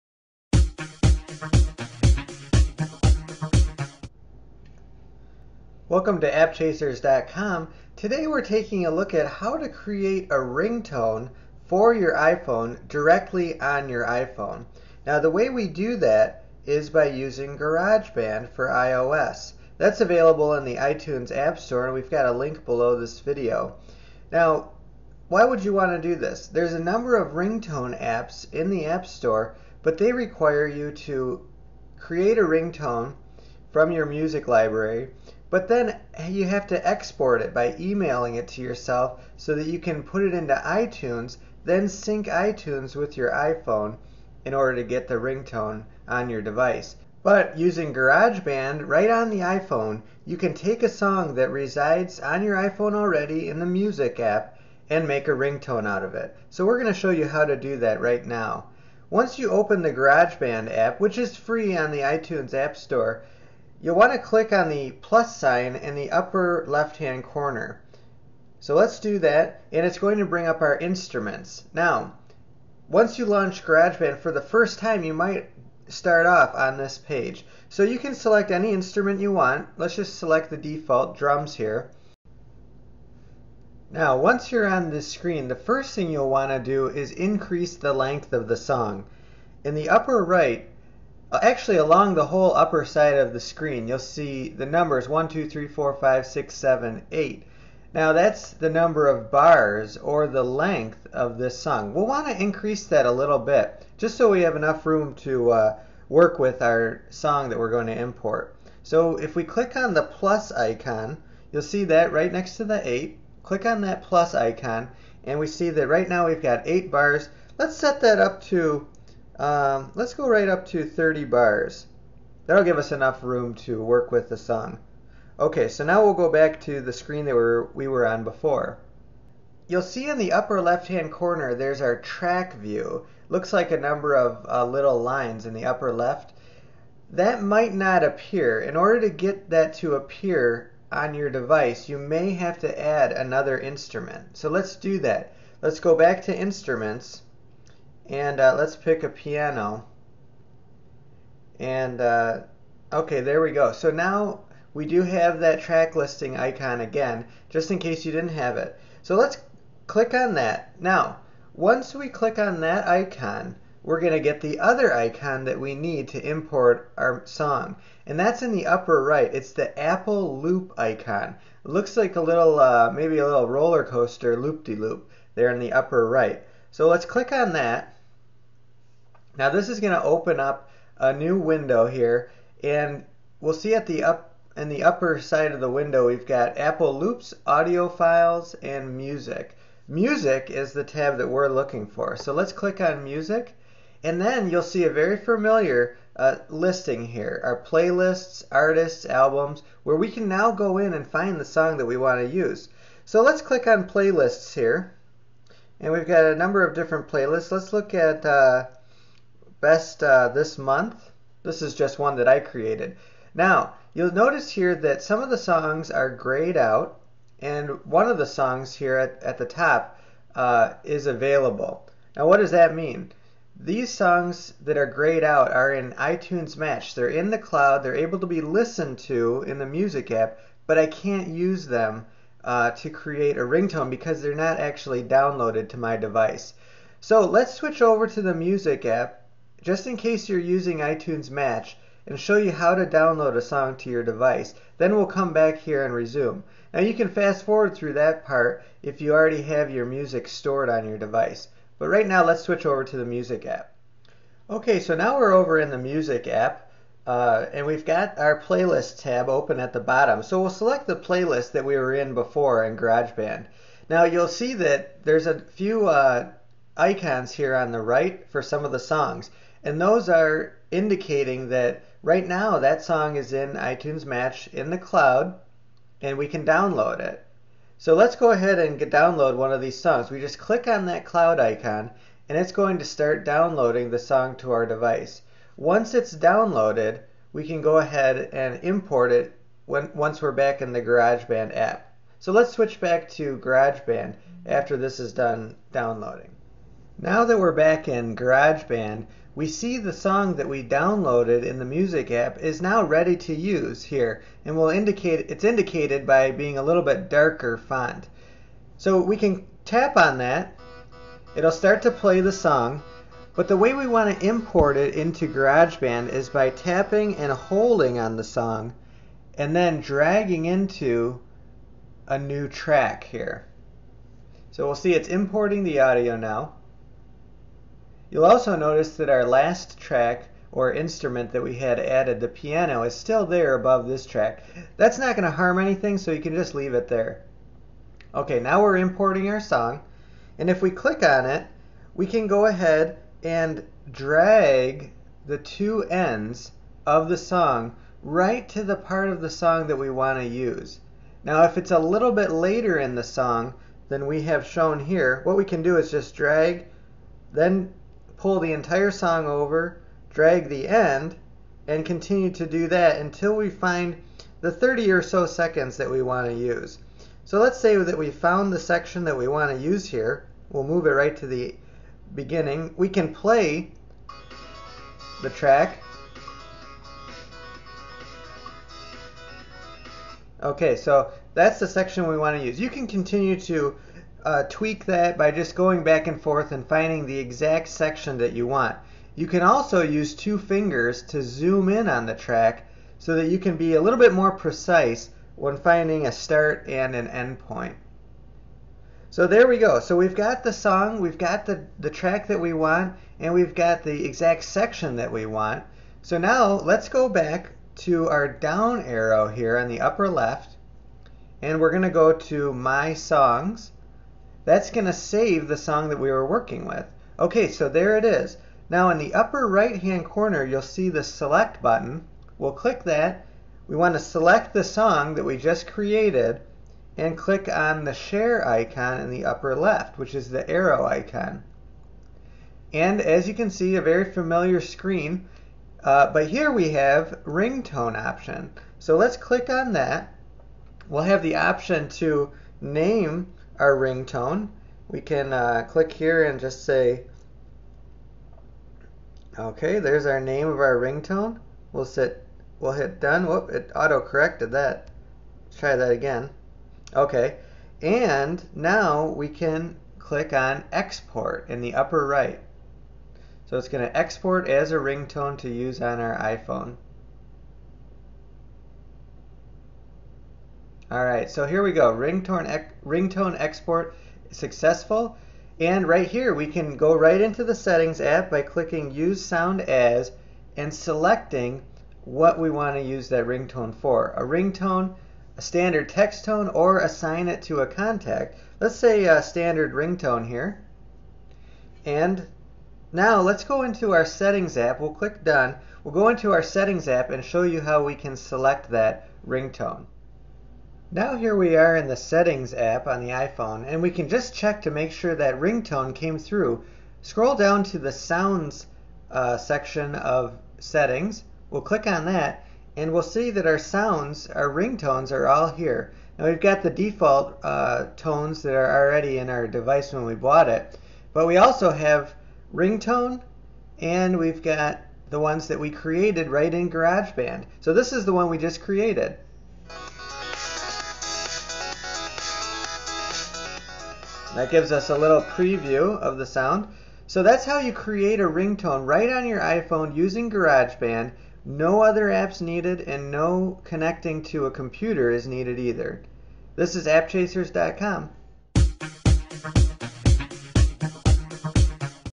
How to Create iPhone Ringtones for Free without iTunes or Jailbreaking
In the video we show you how you can create ringtones right on your iPhone using GarageBand for iOS. No extra apps are required. No computer syncing to iTunes is needed. You can do this all right from your iPhone or iPad